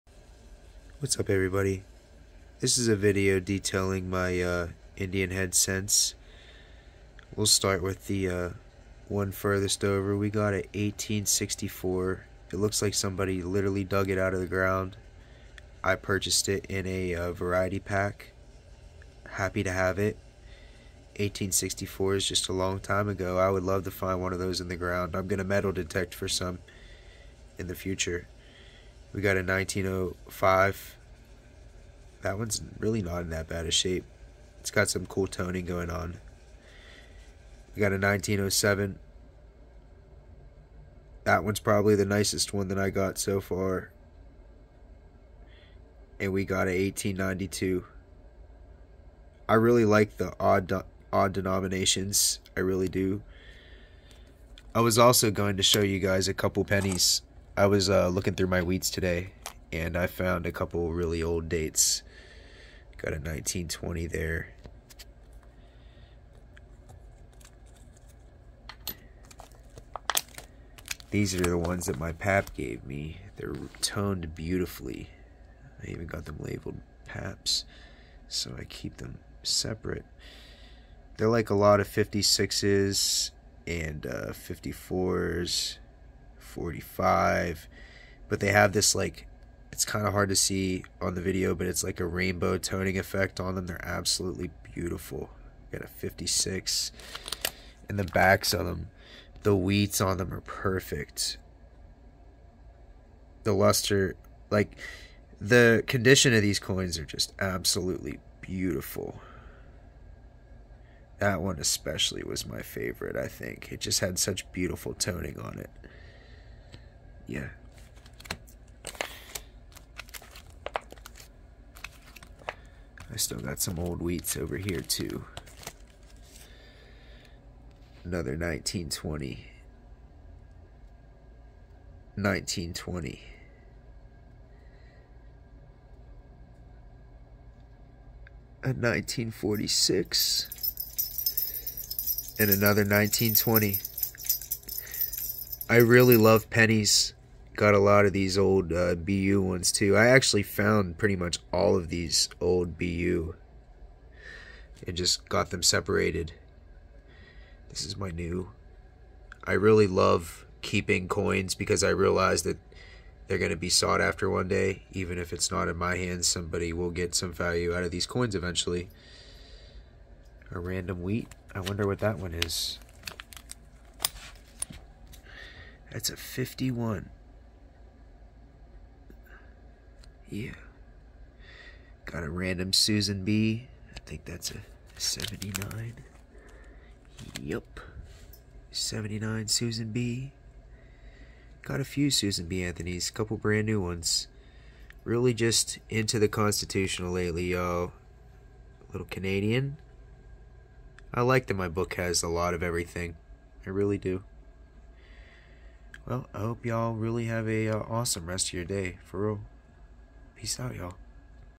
a BEAUTIFUL Wheat Penny & some Indian Head Cents
this video is just a random run thru of some of my favorites! pop in the comments and say hello if you please